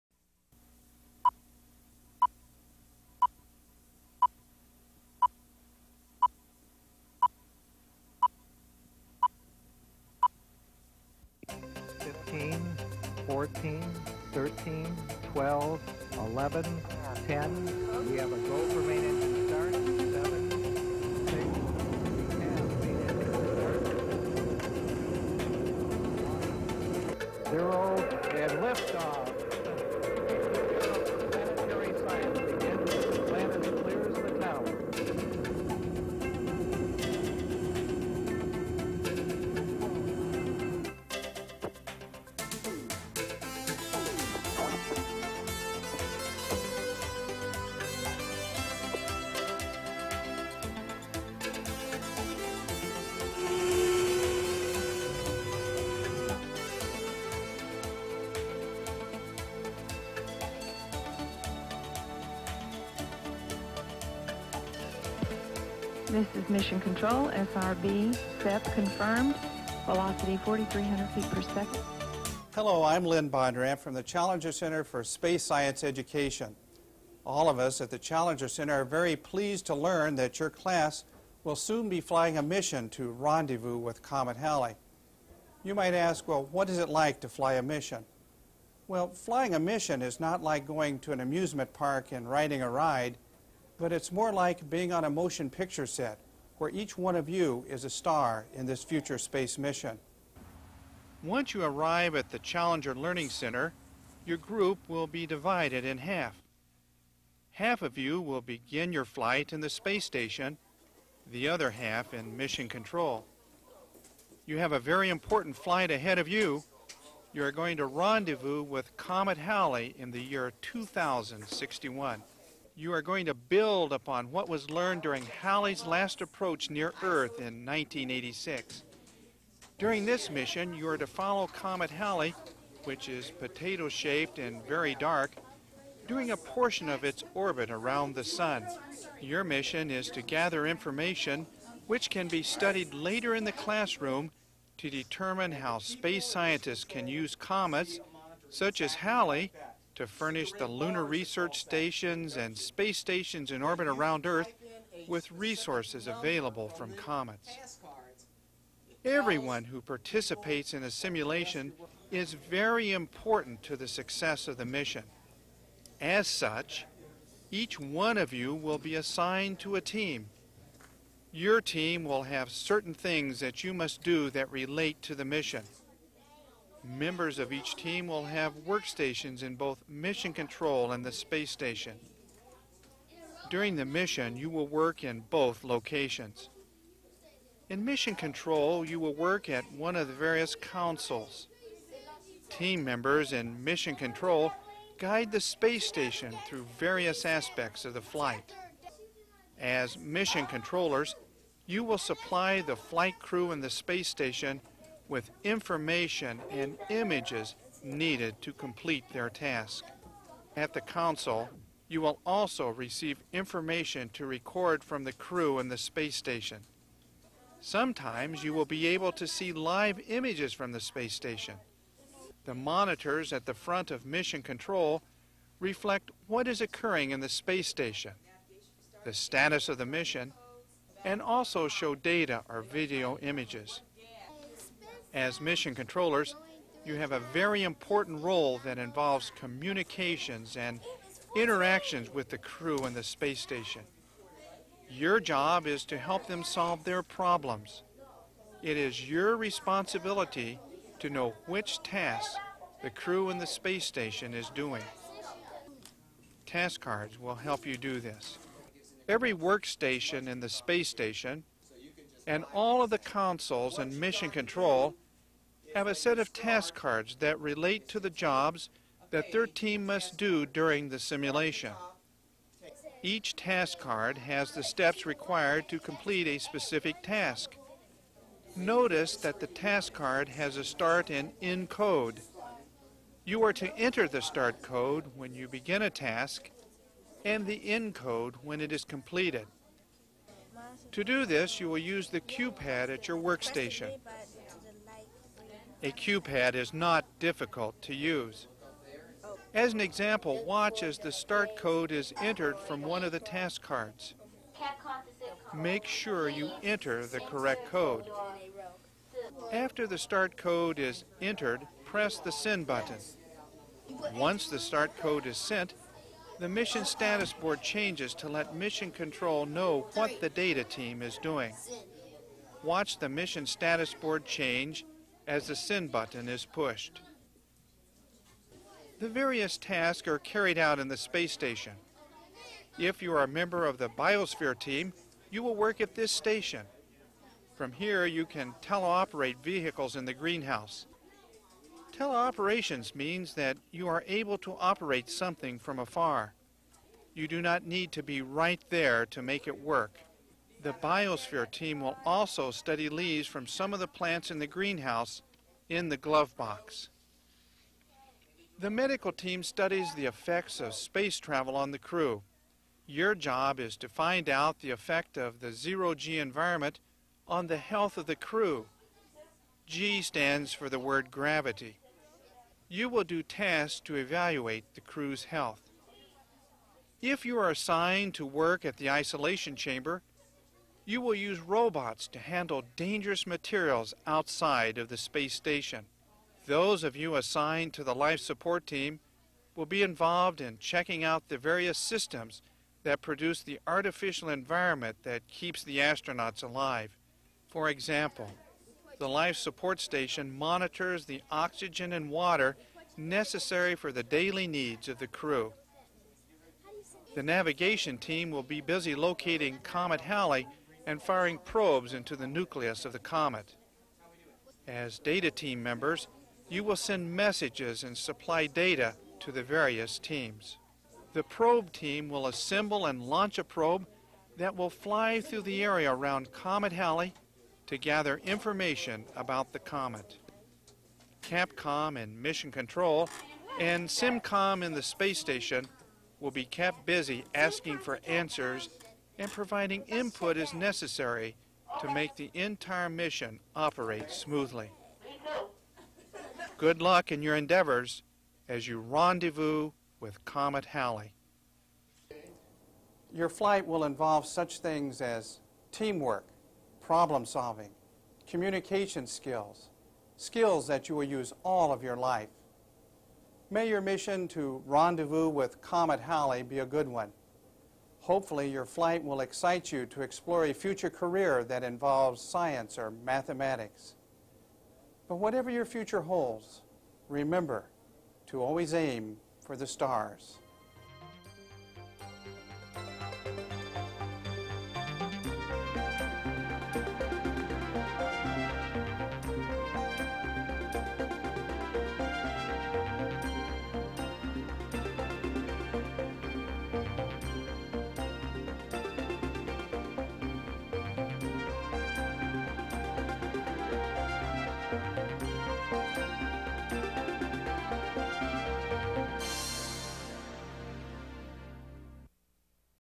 Challenger Center: Rendezvous with Comet Halley No. 3072
This presentation introduces the Challenger Center and the rendezvous with Comet Halley in the 2061 scenario. Dec. 1989